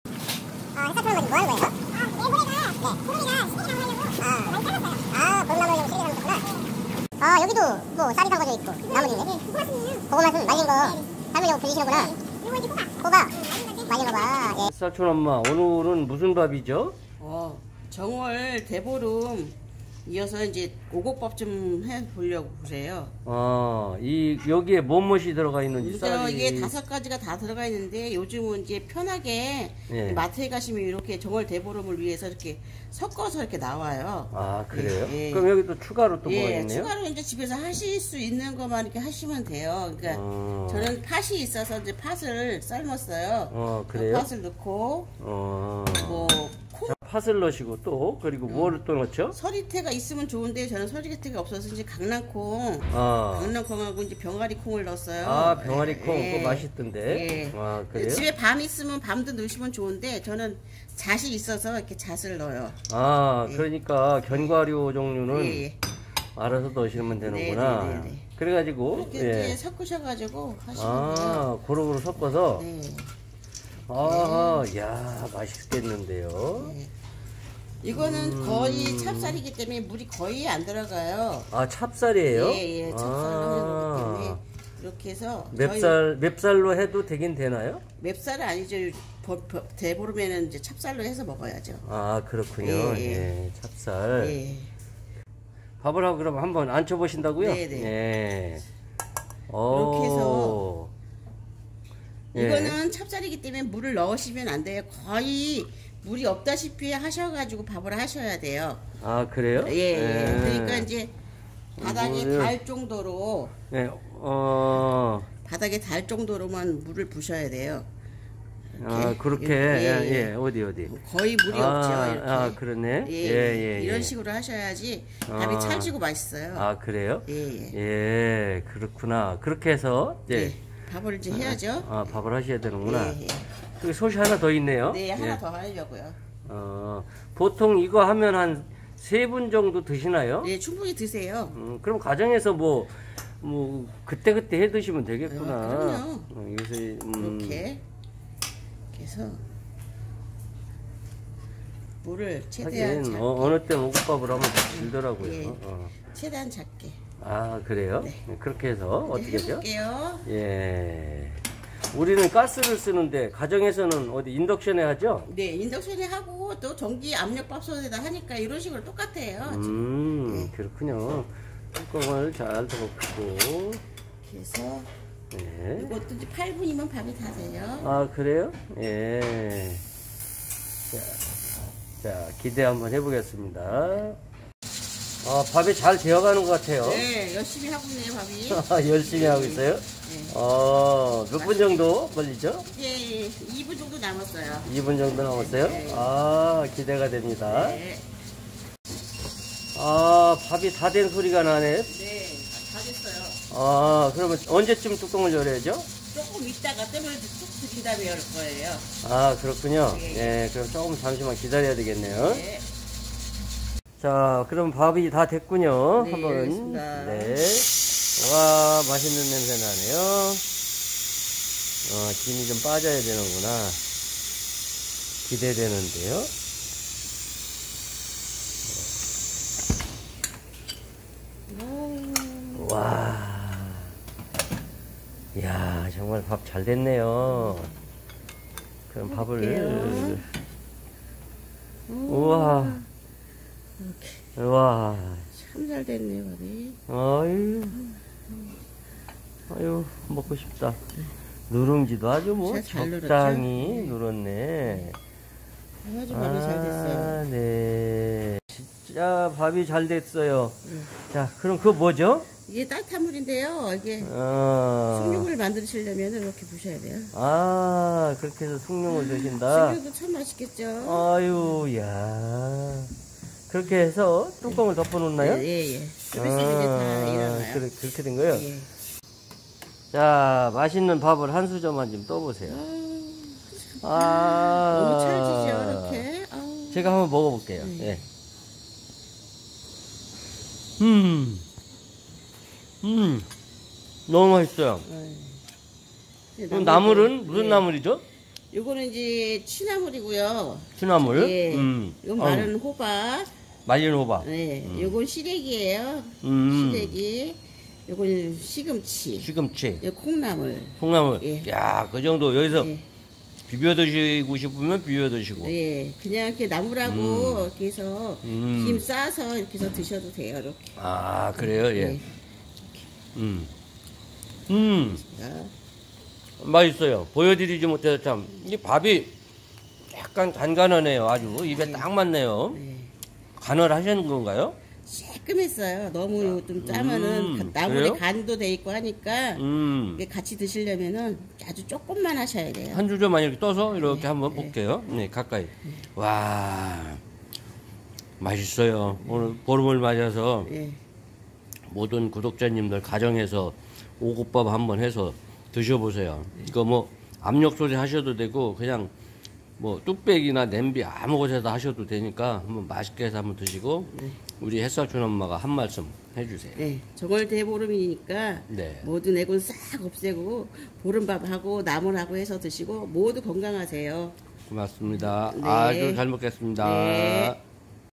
오곡밥만드는법 30년 실무경력 햇살촌 엄마가 오곡밥 쉽고 간단히 만드는법 따라 해보세요.
햇살촌 엄마가 오곡밥 만드는법 따라해보세요